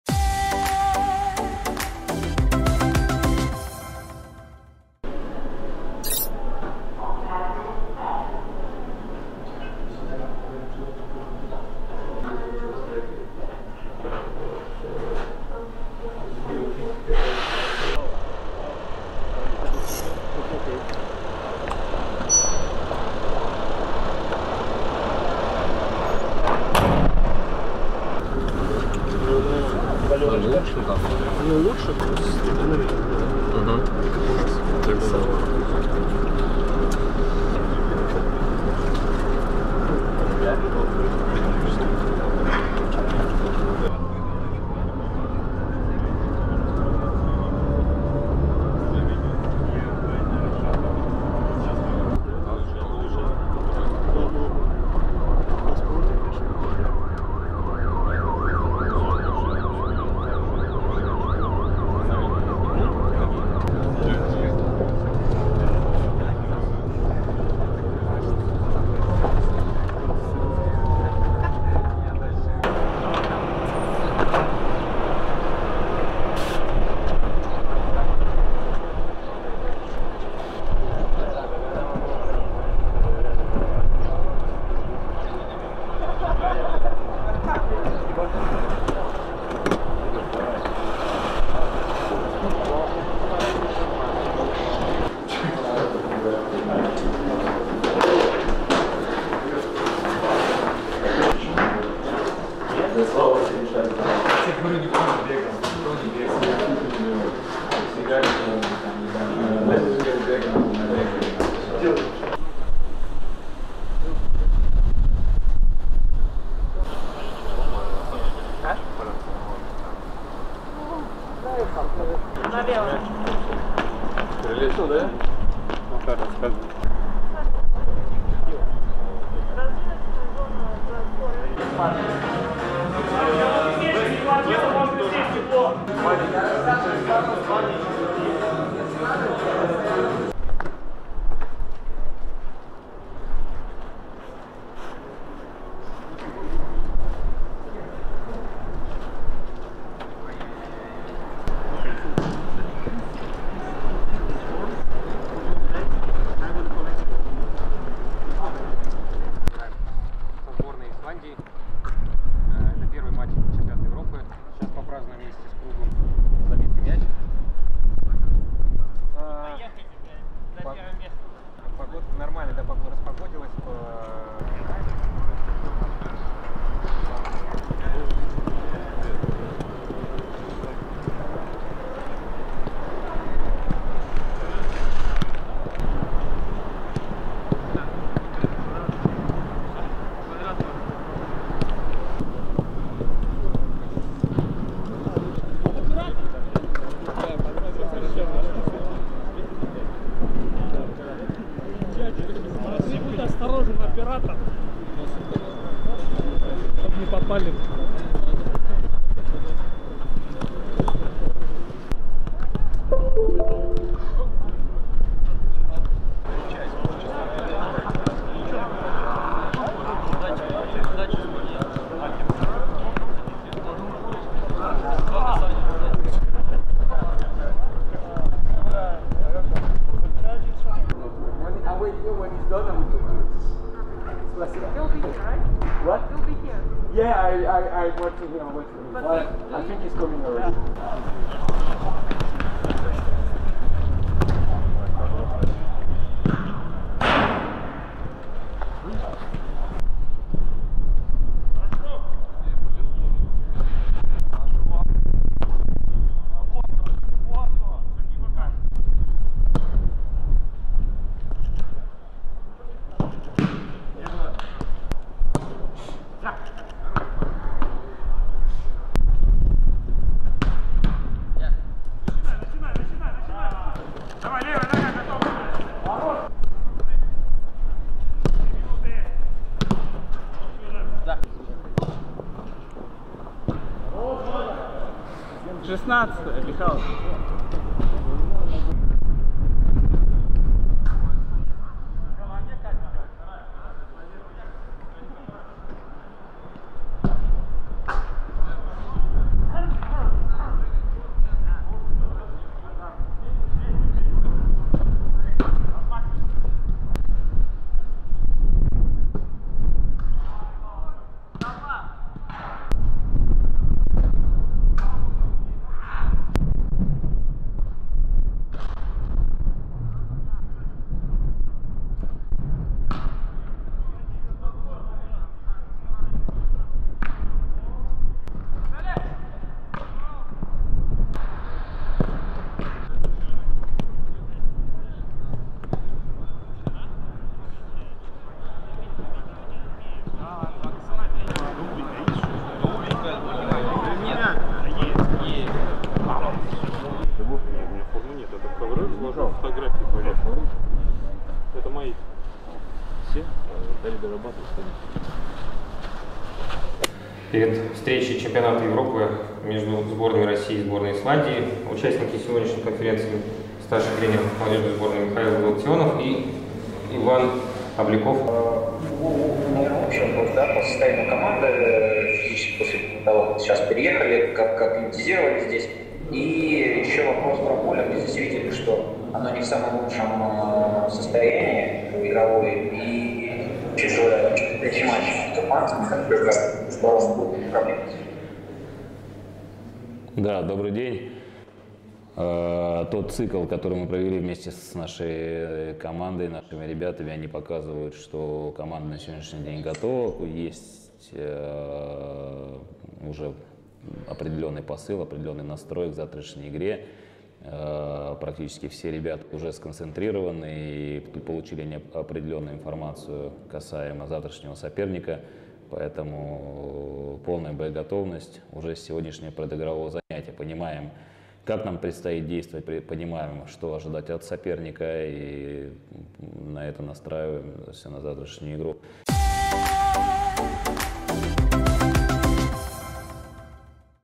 Молодёжка. День 7. Венгрия
Молодёжка провела предигровую тренировку перед матчем с Исландией на основном стадионе "Дьирмот" в г. Дьёр.